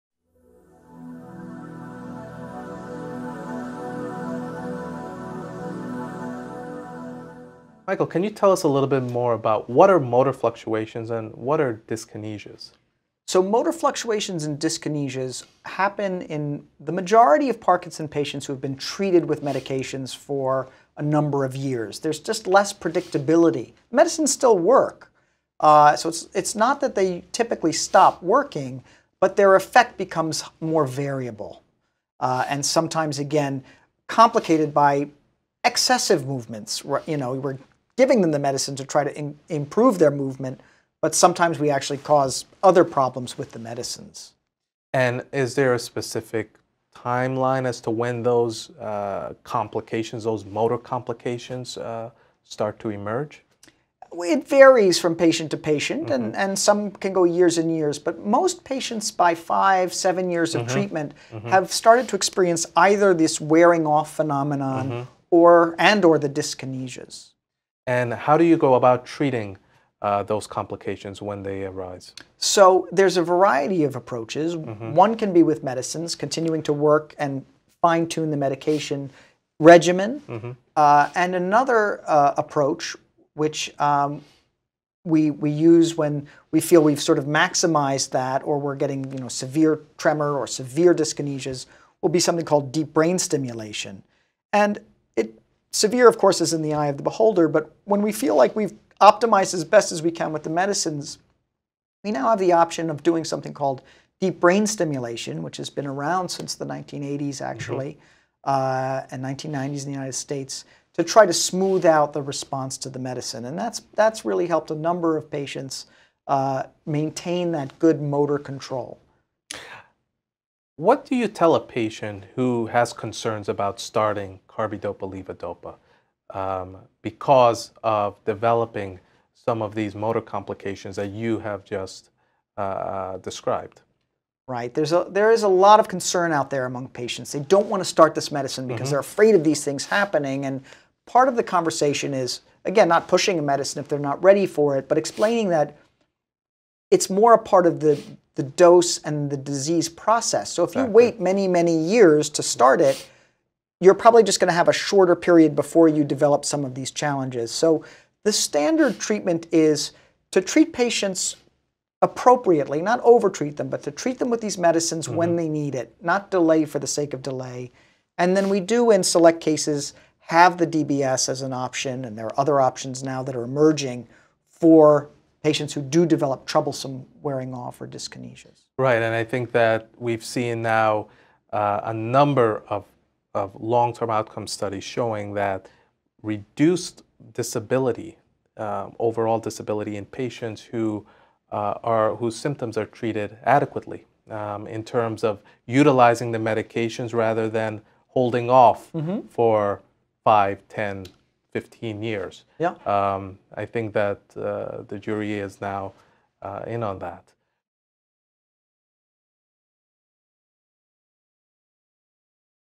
What are motor fluctuations and dyskinesias in Parkinson's disease?
Expert neurologists describe what motor fluctuations and dyskinesias are, and that they happen in patients who’ve been treated with Parkinson’s medications for a number of years. They also talk about how motor fluctuations and dyskinesias can be treated.